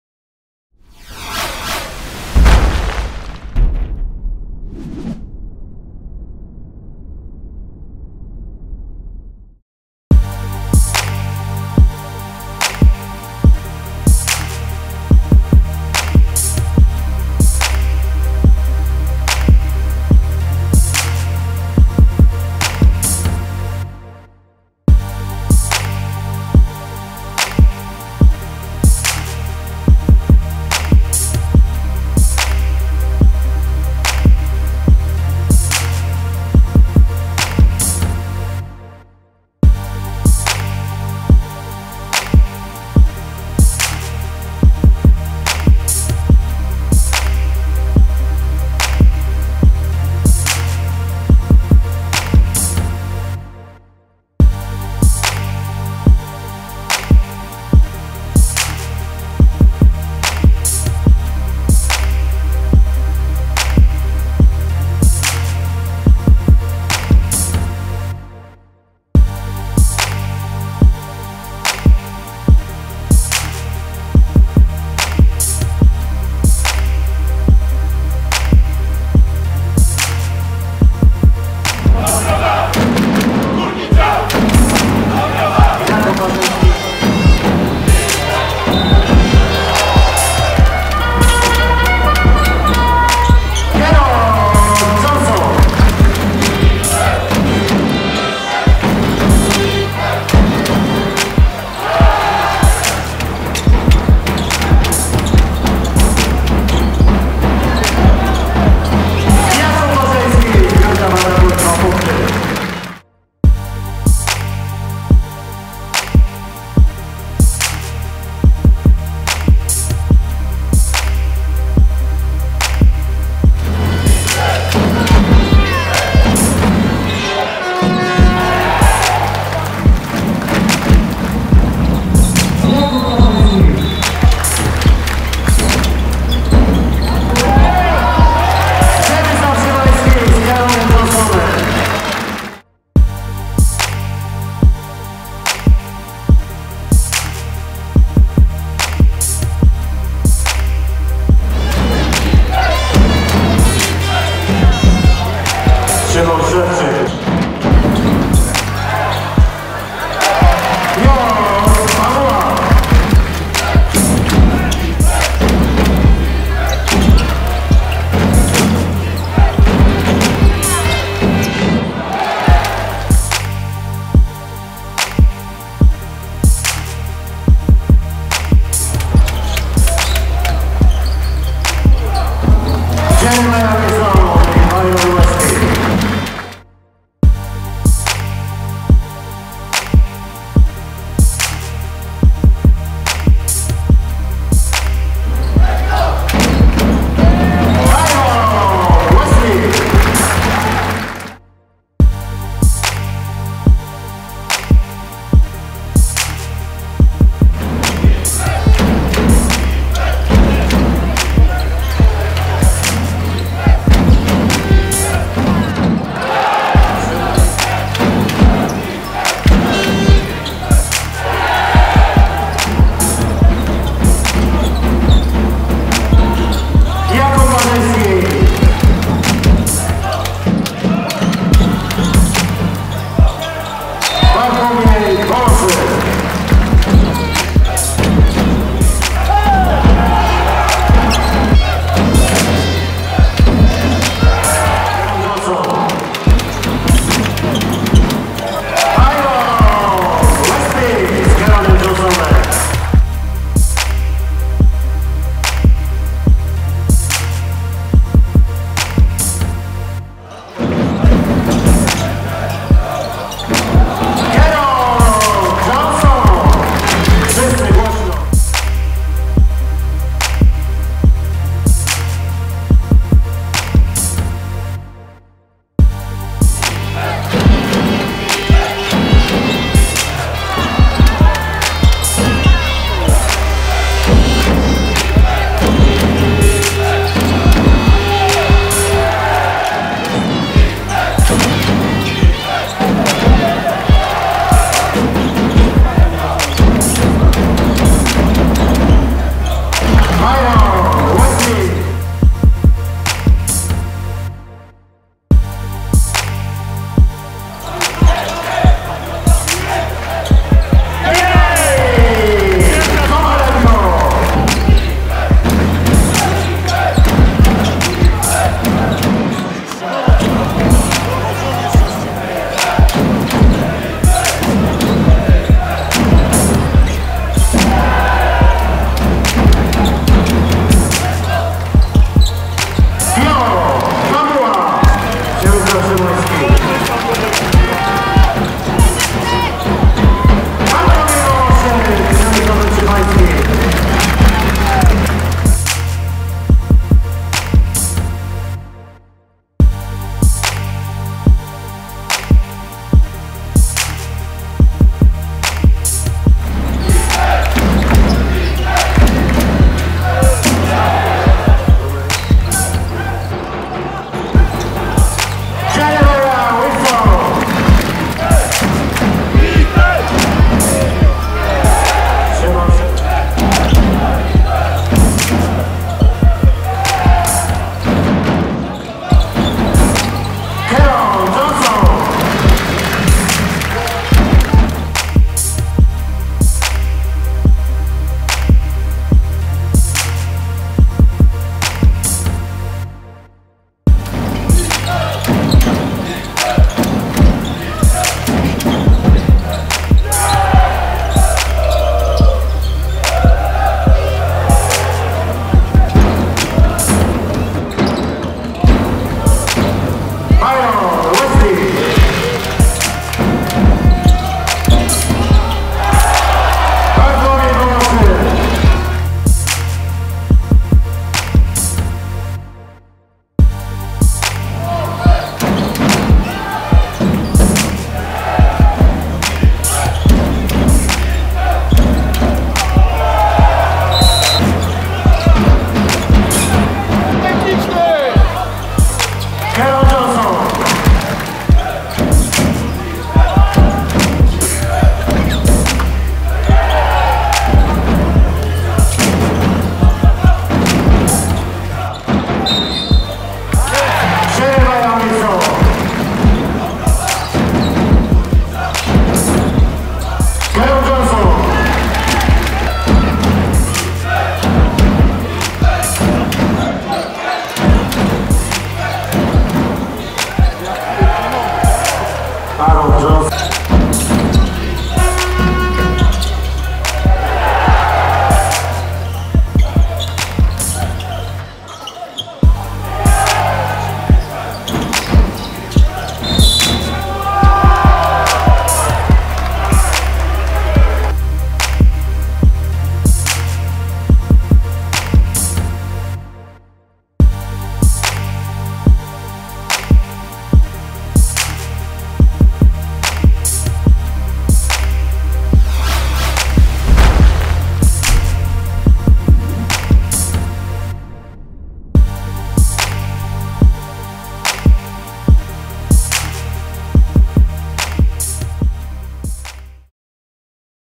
Skrót meczu MKS Dąbrowa Górnicza vs BM Slam Stal Ostrów Wielkopolski.
Mecz odbył się w Dąbrowie Górniczej w Hali Centrum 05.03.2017 r.
Wynik meczu to 61:78 dla BM Slam Stal Ostrów Wielkopolski.